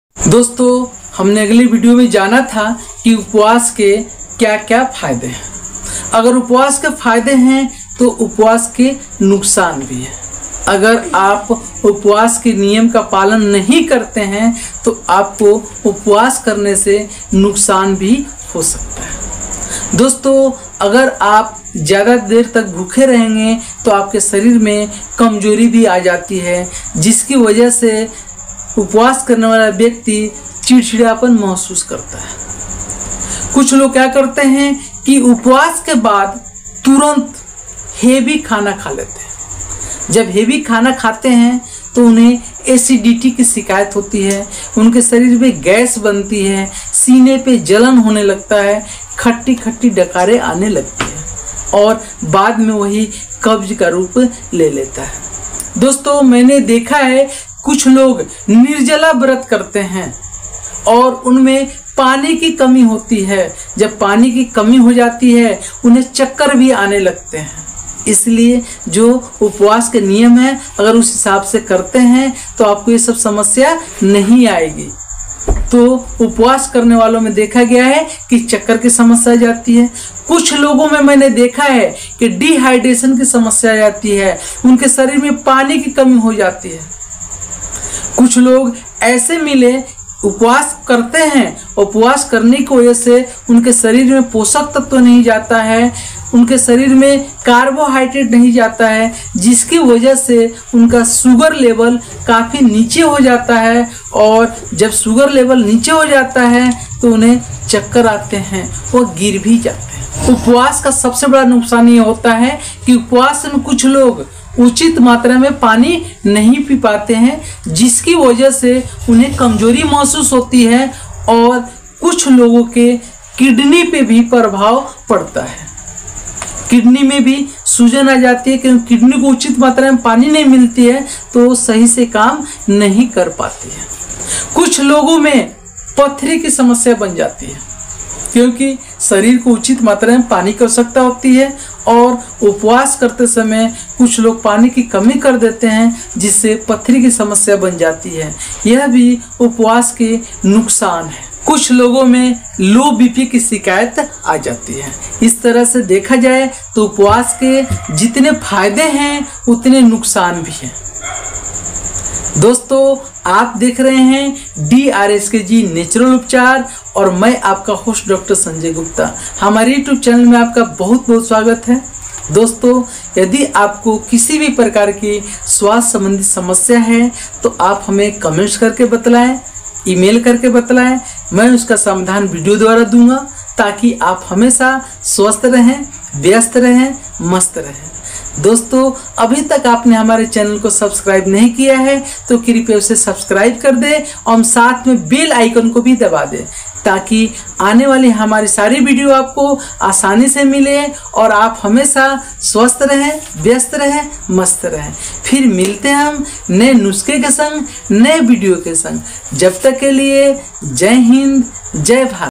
उपवास के नुकसान । Disadvantages of fasting । By Dr. Sanjay Gupta । DrSKG Natural Upchar ।
कुछ या सभी भोजन, पेय या दोनो के लिये बिना कुछ अवधि तक रहना उपवास कहलाता है। उपवास पूर्ण या आंशिक हो सकता है। यह बहुत छोटी अवधि से लेकर महीनो तक का हो सकता है। उपवास के अनेक रूप हैं। धार्मिक एवं आध्यात्मिक साधना के रूप में प्रागैतिहासिक काल से ही उपवास का प्रचलन है। उपवास के अनेकों फायदे होते है, जिससे शरीर् स्वस्थ रहता है एवं अनेक रोगों का नाश होता है ,लेकिन् कभी - कभार इसे तरीके से नही किया जाय तो यह नुकसान भी कर सकता है। आइये जानते है इस वीडियो के जरिये।

Abstaining from some or all food, drink or both for a certain period of time is called fasting. Fasting can be complete or partial. It can be of very short duration to months. There are many forms of fasting. Fasting has been practiced since prehistoric times as a religious and spiritual practice. There are many benefits of fasting, due to which the body remains healthy and many diseases are destroyed, but sometimes if it is not done properly, it can also cause harm. Let's know through this video.